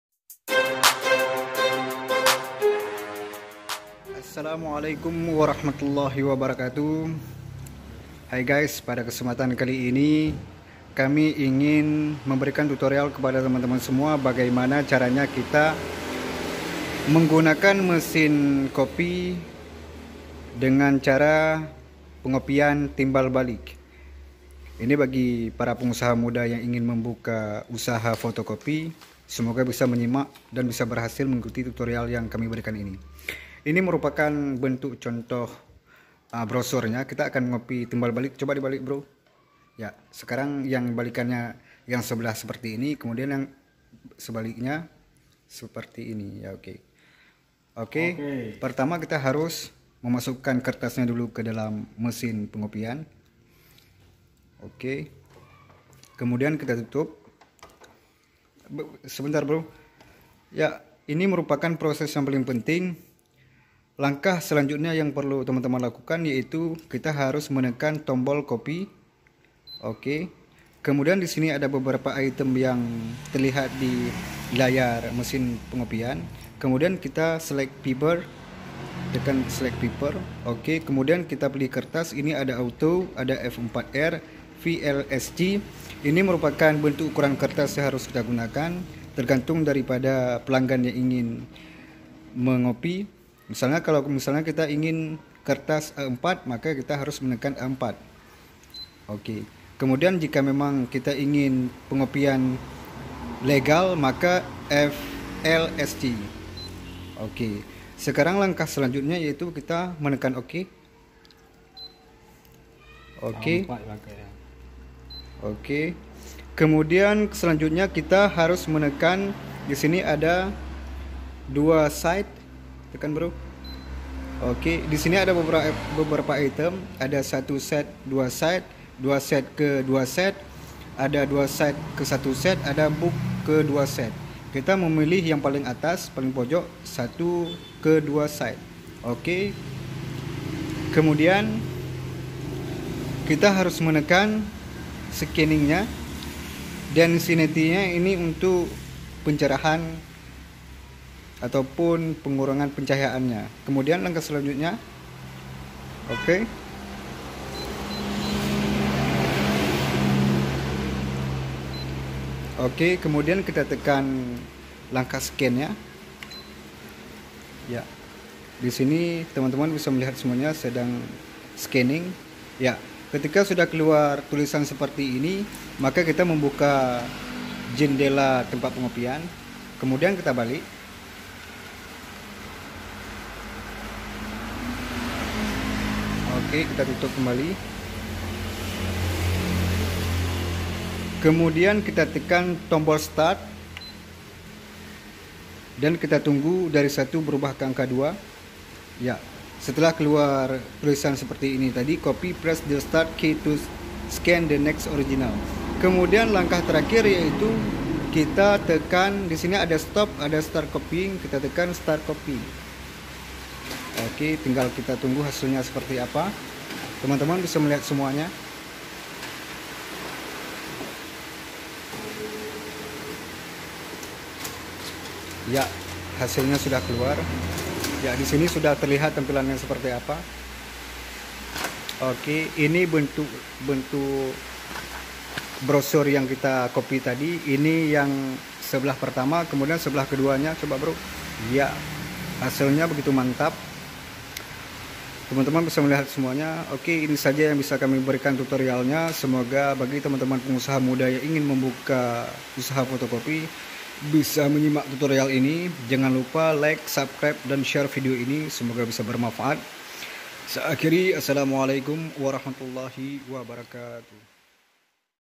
Cara Mengopy Timbal Balik Menggunakan Mesin Fotocopy
USAHA FIKOH MAKIN MAJU - BEGINI CARA MENGOPY KERTAS TIMBAL BALIK DI MESIN FOTOCOPY

Dalam video ini memberikan tutorial bagaimana cara mengopy kertas timbal balik versi usaha fikoh. Bagi teman-teman yang ingin membuka usaha fotocopy turorial ini sangat bermamfaat agar hasil fotocopy timbal balik menjadi bagus dan maksimal.